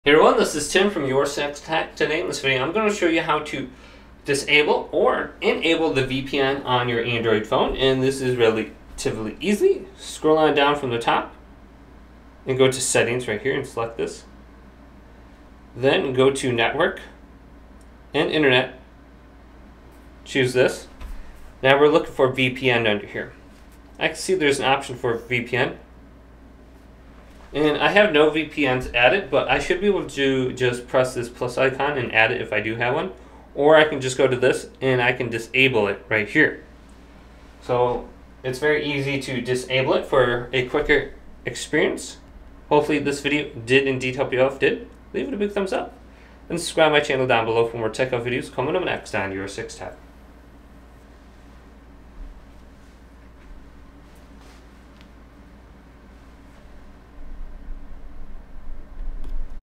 How to Turn On or OFF the VPN on your Android Phone (Settings Tutorial)
Turning off or on a VPN on your Android phone is straightforward. Here are the steps:

Using the Android Settings:
Open your Settings app.
Navigate to Network & internet.
Tap on VPN.

Find the VPN connection you want to disable and toggle it off.
Using the VPN App:

If you have a VPN app installed, open it.

Look for an option to disconnect or turn off the VPN.

Remember that keeping your VPN active is generally recommended for privacy and security. However, there might be scenarios where you need to disable it temporarily, such as troubleshooting network issues or accessing services available only in your country. Just be cautious and remember to turn it back on when needed!